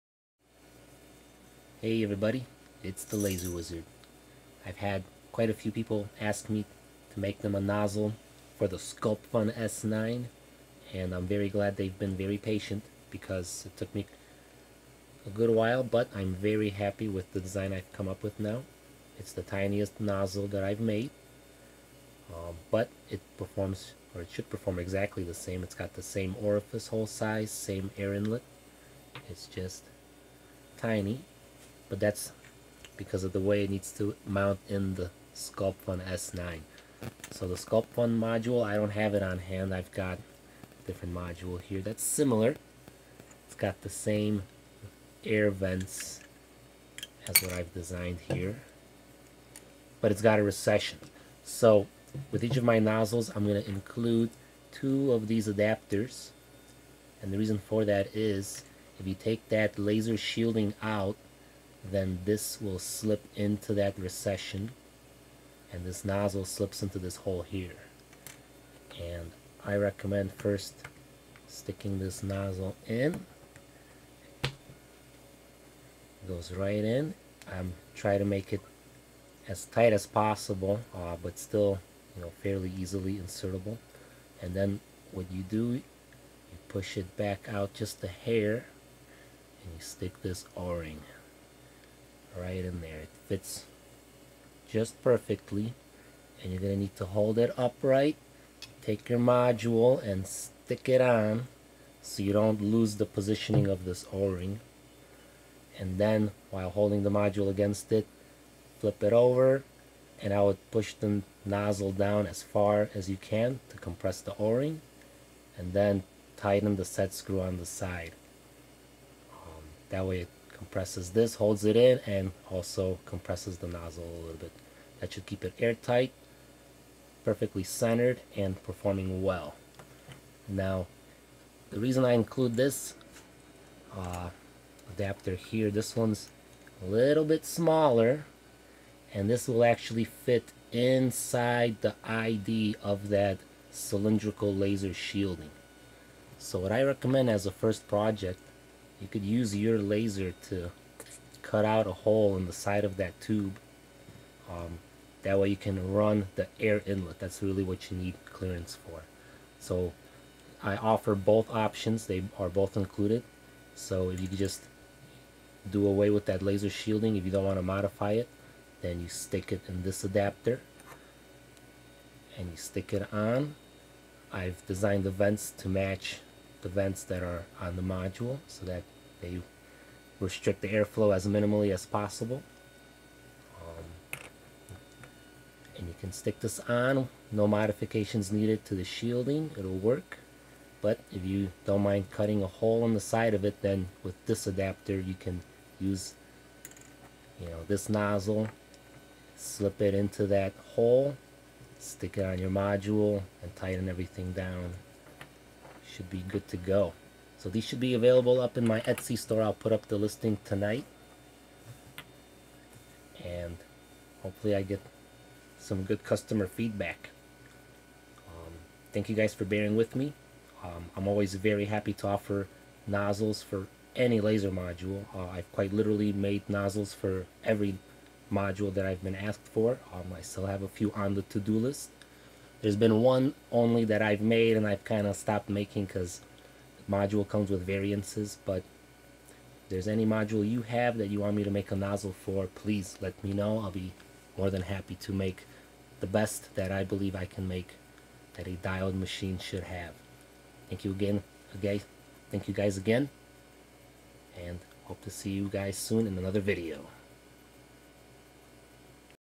Sculpfun S9 Air Assist Nozzle
This is a quick video I shot to show my latest nozzle variant. I have had a few people ask me to make one for the Sculpfun S9. This one took a while to come up with a solution that I was happy with, but I am happy with it so far. I hope that you too might be happy with it as well!

Please let me know what you think and if you have any suggestions! You can find this nozzle and all my other ones listed on our etsy page located here:

THE O-RING IS NOT NECESSARY AND IS NO LONGER INCLUDED!!!

This nozzle comes with two acrylic adapter plates to fit within the 32mm wide and 6mm deep recession of the S9 module (Be sure yours is not a different variant, check the photos I posted and the dimensions). One of the adapter plates is smaller to fit inside the ID of the cylindrical laser shield. You can still use your laser shielding with this nozzle if you are willing to drill a hole in the side of the shield to let the air inlet through. A great first project with the nozzle would be to use the larger adapter plate to install the nozzle and use it with air assist to cleanly cut a hole in the side of the laser shield tube (if you are experienced and confident enough!), otherwise, you could drill it with a spiral drill (ideally a dulled drill bit).